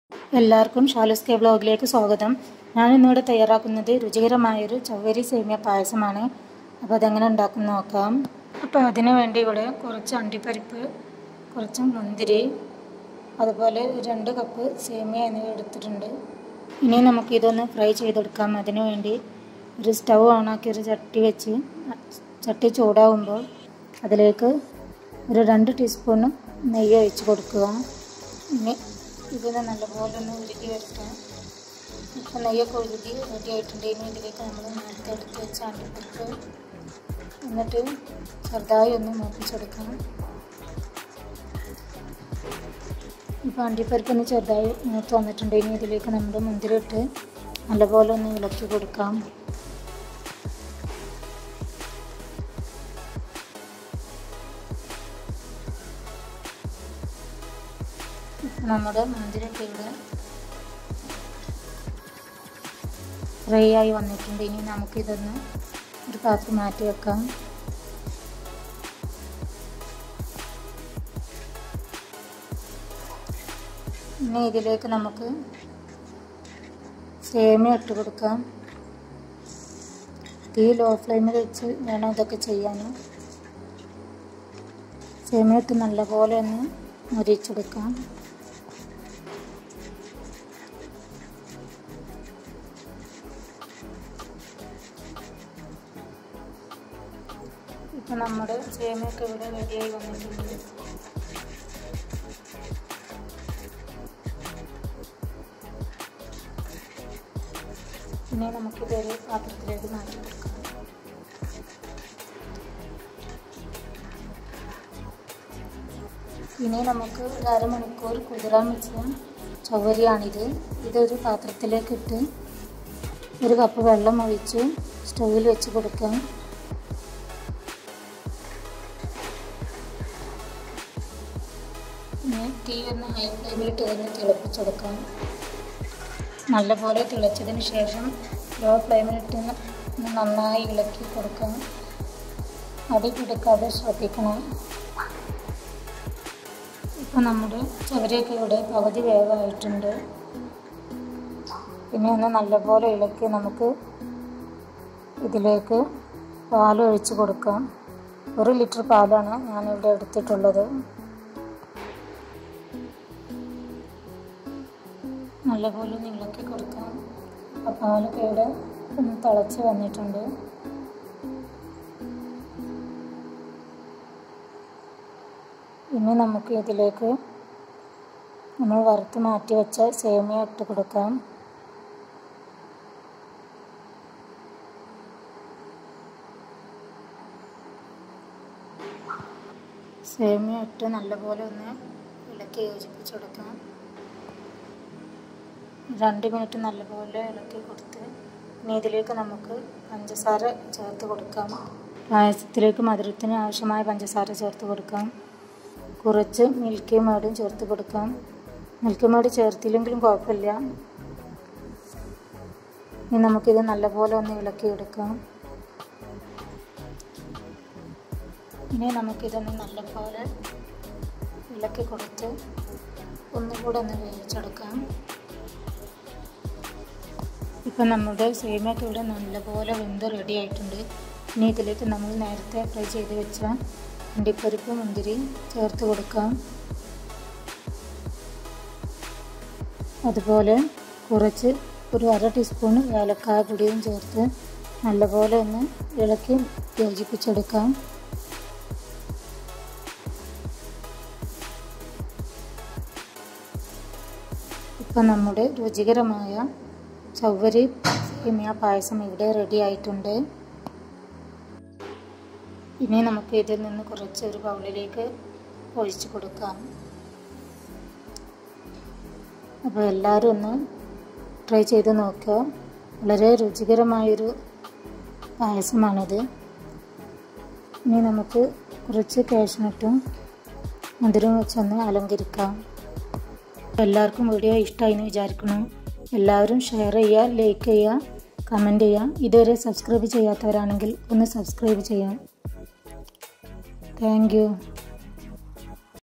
സൂപ്പർ ടേസ്റ്റിൽ ഈസിയായി ചവ്വരി സേമിയ പായസം //choweri semia payasam 😋😋
ചവ്വരി -1കപ്പ്
സേമിയ -1കപ്പ്
നെയ്യ് -2ടിസ്പൂൺ
അണ്ടിപ്പരിപ്പ് -ആവശ്യത്തിന്
ഉണക്ക മുന്തിരി -ആവശ്യത്തിന്
പഞ്ചസാര -ആവശ്യത്തിന്
ഏലക്കാപ്പൊടി -അര ടിസ്പൂൺ